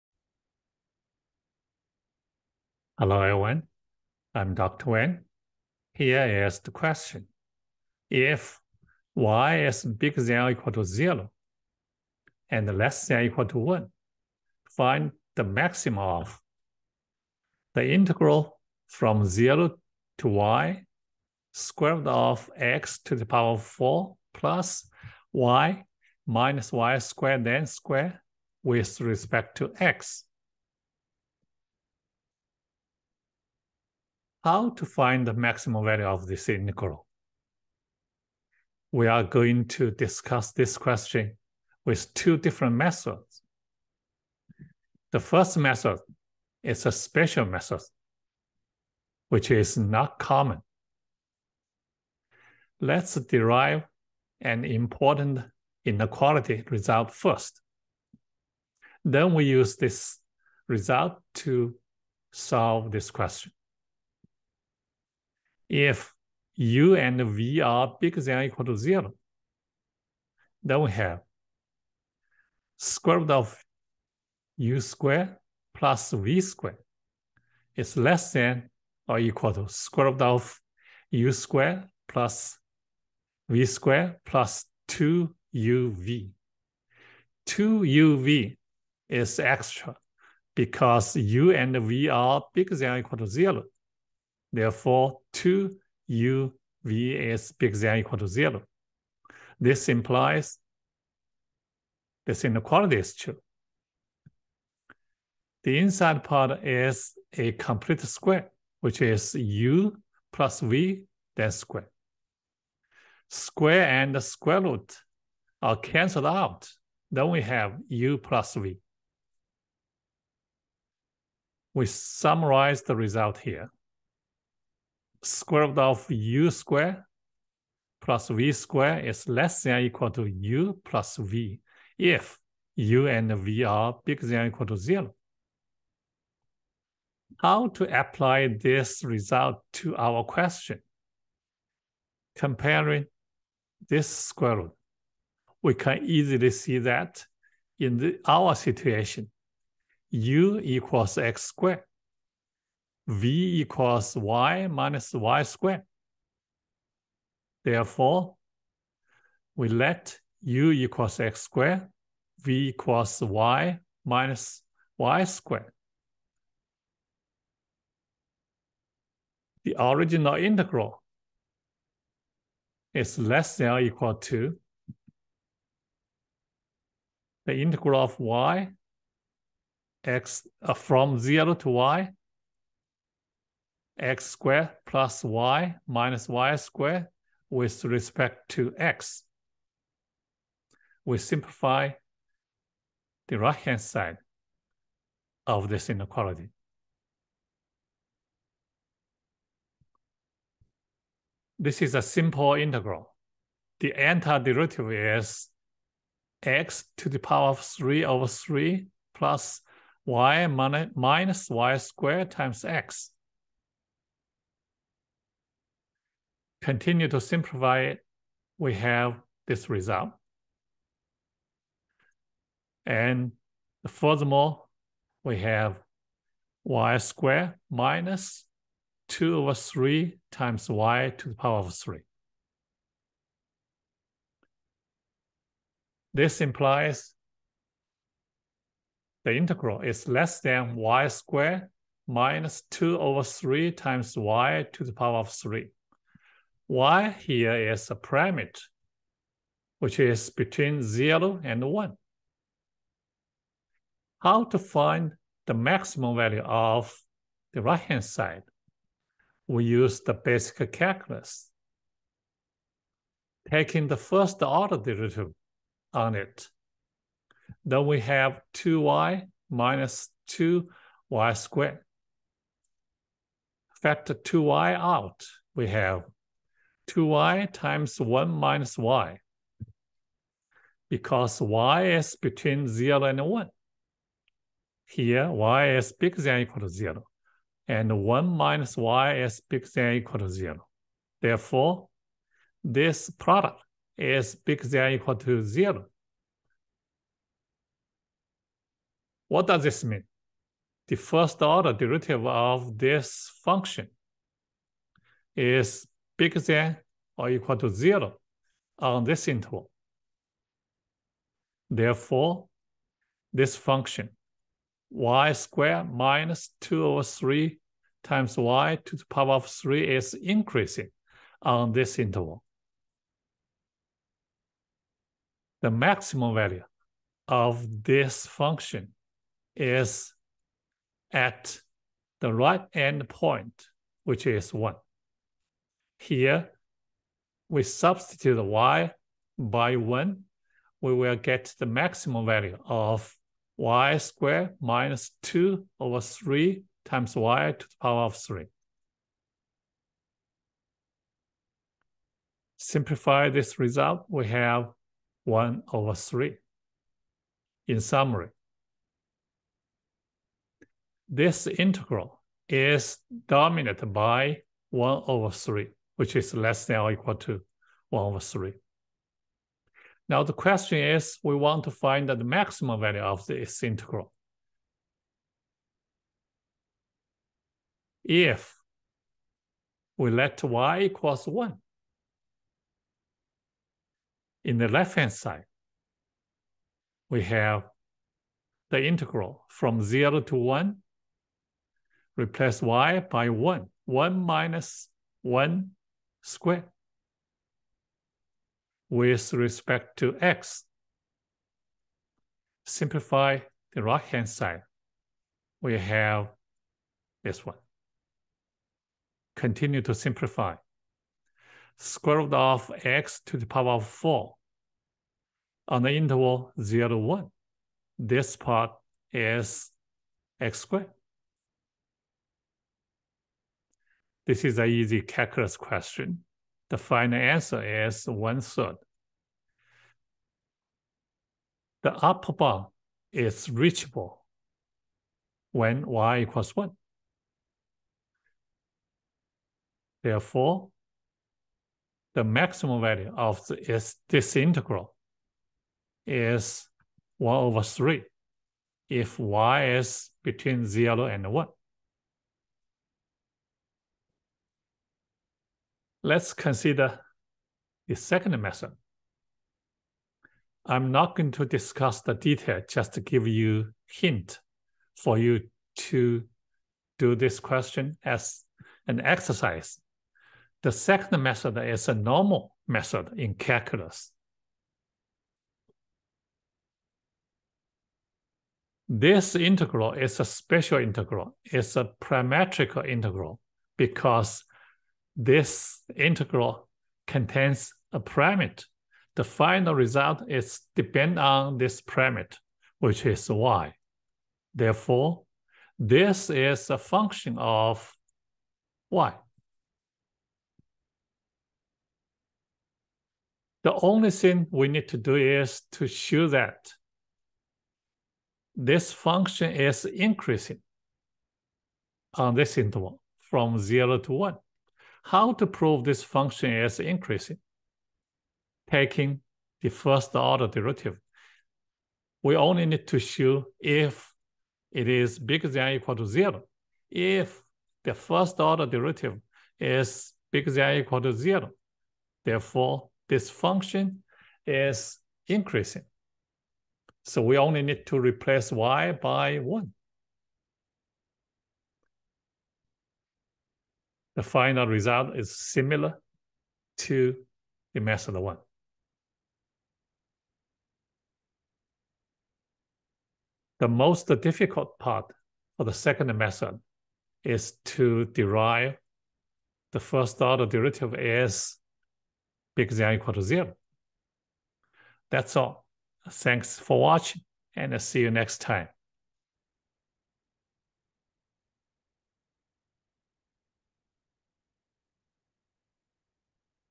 Putnam Math Competition Question | Find the maximum of this integral as function of y | Calculus
Can you find the maximum value of this integral which is a function of y? This is a Putnam Mathematical Competition question in calculus. We'll discuss two methods to solve this problem. The first one is a special method. The second one is a common method.